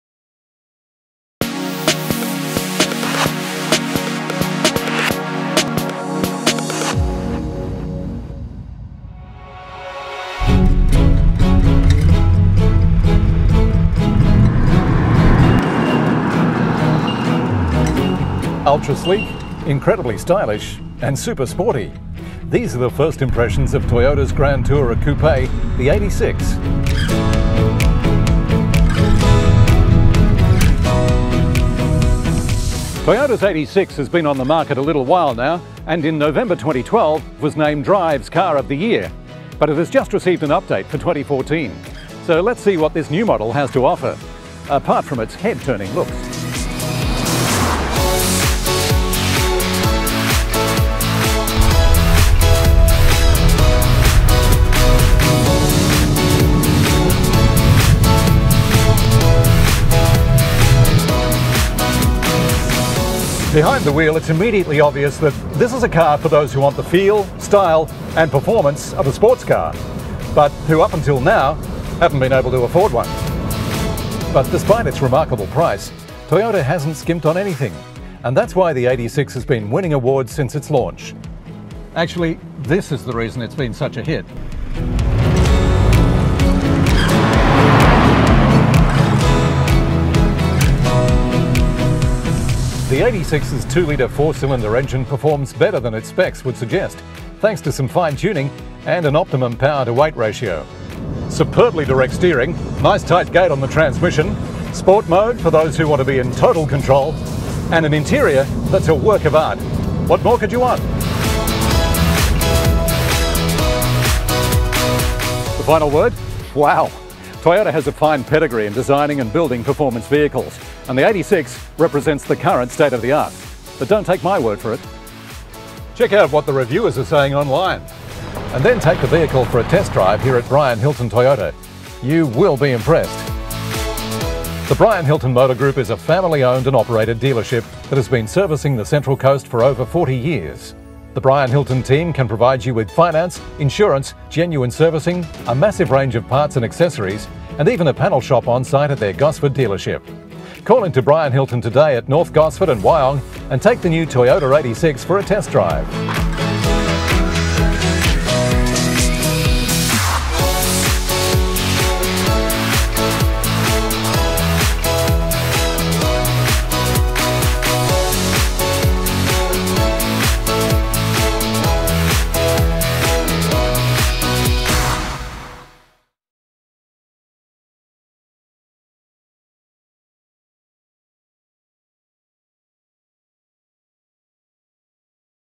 2014 Toyota 86 update review - Brian Hilton Toyota
The updated Toyota 86 is available now at Brian Hilton Toyota, North Gosford and Wyong.